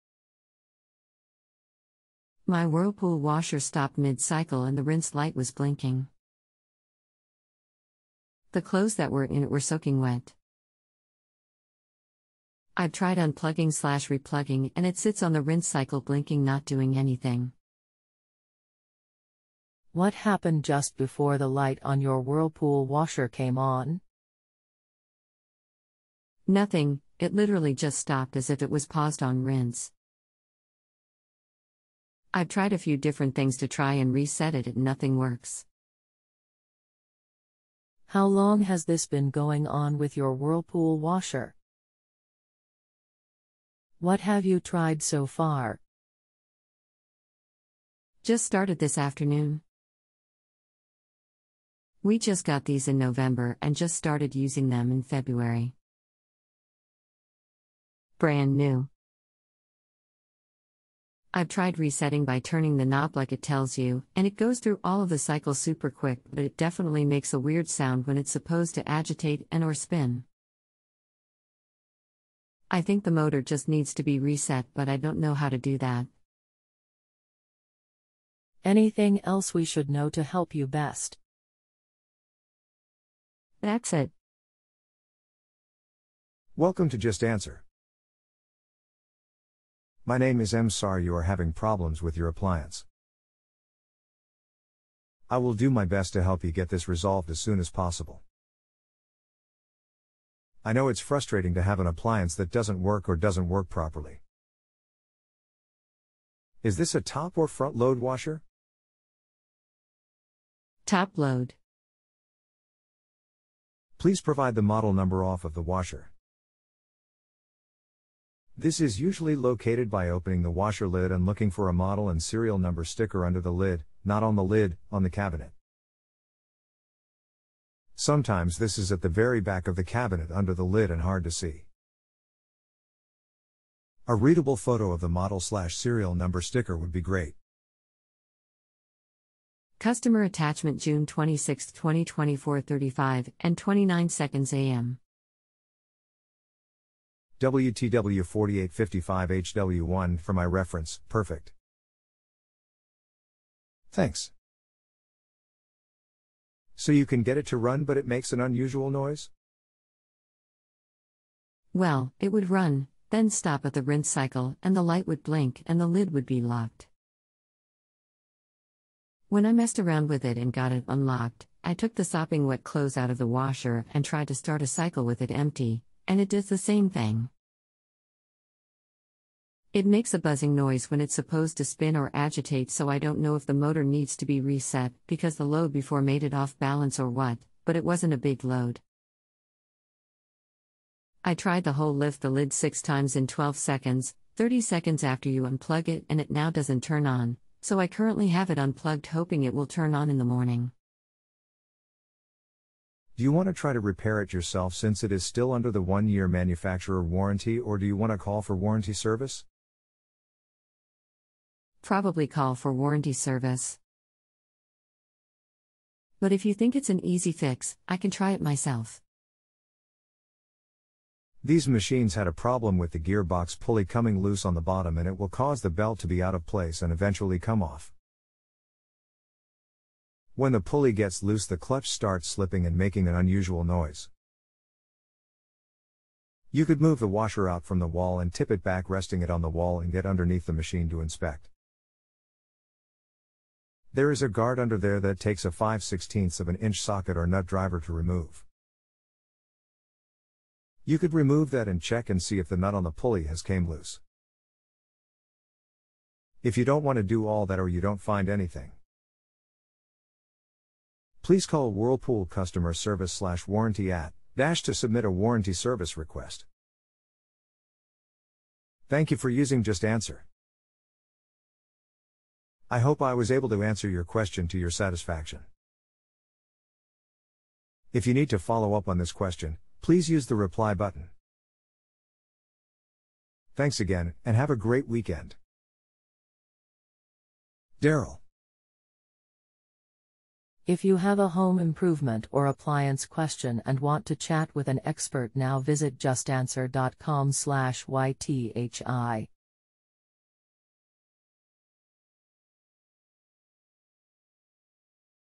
My whirlpool washer stopped mid cycle and the rinse light
JustAnswer Customer: My whirlpool washer stopped mid cycle and the rinse light was blinking.
JustAnswer Customer: The clothes that were in it were soaking wet.
JustAnswer Customer: I’ve tried unplugging/replugging and it sits on the rinse cycle blinking not doing anything
Pearl Wilson: Assistant: What happened just before the light on your Whirlpool washer came on?
JustAnswer Customer: Nothing, it literally just stopped as if it was paused on rinse.
JustAnswer Customer: I’ve tried a few different things to try and reset it and nothing works
Pearl Wilson: Assistant: How long has this been going on with your Whirlpool washer?
JustAnswer Customer: just started this afternoon.
JustAnswer Customer: We just got these in November and just started using them in February.
JustAnswer Customer: Brand new.
JustAnswer Customer: I’ve tried resetting by turning the knob like it tells you, and it goes through all of the cycle Super quick but it definitely makes a weird sound when it’s suppose to agitate and/or spin.
JustAnswer Customer: I think the motor just needs to be reset but I don’t know how to do that
JustAnswer Customer: thats it
Darrel G: Home Appliance Technician: Welcome to JustAnswer!
Darrel G: Home Appliance Technician: My name is*'m sorry you are having problems with your appliance.
Darrel G: Home Appliance Technician: I will do my best to help you get this resolved as soon as possible.
Darrel G: Home Appliance Technician: I know it’s frustrating to have an appliance that doesn’t work or doesn’t work properly.
Darrel G: Home Appliance Technician: *Is this a top or front load washer?
JustAnswer Customer: Top load
Darrel G: Home Appliance Technician: Please provide the model number off of the washer.
Darrel G: Home Appliance Technician: This is usually located by opening the washer lid and looking for a model and serial number sticker under the lid (not on the lid) on the cabinet.
Darrel G: Home Appliance Technician: Sometimes this is at the very back of the cabinet under the lid and hard to see.
Darrel G: Home Appliance Technician: A readable photo of the model/serial number sticker would be great.
JustAnswer Customer: Customer attachment 6/26/2020 4:35:29 AM
Darrel G: Home Appliance Technician: WTW4855HW1  (for my reference) Perfect.
Darrel G: Home Appliance Technician: Thanks.
Darrel G: Home Appliance Technician: So you can get it to run but it makes an unusual noise?
JustAnswer Customer: Well, it would run, then stop at the rinse cycle and the light would blink and the lid would be locked.
JustAnswer Customer: When I messed around with it and got it unlocked, I took the sopping-wet clothes out of the washer and tried to start a cycle with it empty, and it does the same thing.
JustAnswer Customer: It makes a buzzing noise when it's suppose to spin or agitate so i don't know if the motor needs to be reset because the load before made it off balance or what, but it wasn't a big load.
JustAnswer Customer: I tried the whole lift the lid 6 times in 12 seconds, 30 seconds after you unplug it and it now doesn't turn on, so I currently have it unplugged hoping it will turn on in the morning
Darrel G: Home Appliance Technician: Do you want to try to repair it yourself since it is still under the 1 year manufacturer warranty or do you want to call for warranty service?
JustAnswer Customer: Probably call for warranty service
JustAnswer Customer: But if you think it's an easy fix, I can try it myself
Darrel G: Home Appliance Technician: These machines had a problem with the gear box pulley coming loose on the bottom and it will cause the belt to be out of place and eventually come off.
Darrel G: Home Appliance Technician: When the pulley gets loose the clutch starts slipping and making an unusual noise.
Darrel G: Home Appliance Technician: You could move the washer out from the wall and tip it back resting it on the wall and get underneath the machine to inspect.
Darrel G: Home Appliance Technician: There is a guard under there that takes a 5/16" socket or nut driver to remove.
Darrel G: Home Appliance Technician: You could remove that and check and see if the nut on the pulley has came loose.
Darrel G: Home Appliance Technician: If you don't want to do all that or you don't find anything.
Darrel G: Home Appliance Technician: Please call Whirlpool Customer Service/Warranty at  *-*-*  to submit a warranty service request.
Darrel G: Home Appliance Technician: Thank you for using JustAnswer.
Darrel G: Home Appliance Technician: I hope I was able to answer your question to your satisfaction.
Darrel G: Home Appliance Technician: If you need to follow up on this question, please use the reply button.
Darrel G: Home Appliance Technician: Thanks again and have a great weekend!